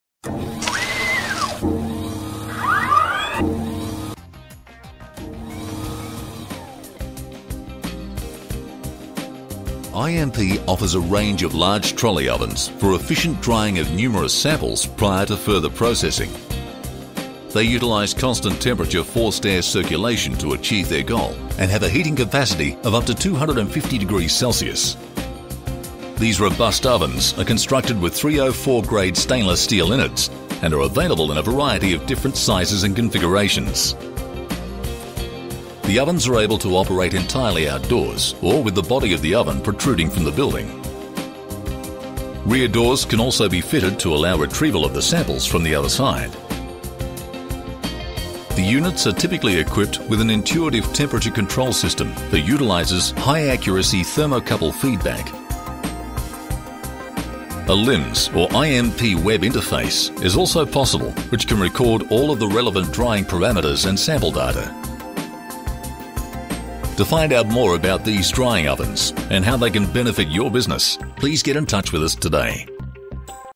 IMP Automation - Large trolley drying ovens
IMP offers a range of large trolley ovens for efficient drying of numerous samples prior to further processing.
They utilize constant temperature forced air circulation to achieve their goal, and have a heating capacity of up to 250 degrees Celsius.
These robust ovens are constructed with 304 grade stainless steel innards, and are available in a variety of different sizes and configurations from 2000 liters to 8000 liters.
Rear doors can also be fitted to allow retrieval of the samples from the other side.
The ovens are able to operate entirely outdoors, or with the body of the oven protruding from the building.
The units are typically equipped with an intuitive temperature control system that utilises high accuracy thermocouple feedback.
An audible alarm and flashing light alerts the operator when the cycle is complete.
A LIMS or IMP web interface is also possible which can record all of the relevant drying parameters and sample data.
To find out more about these drying ovens and how they can benefit your business, please get in touch with us today.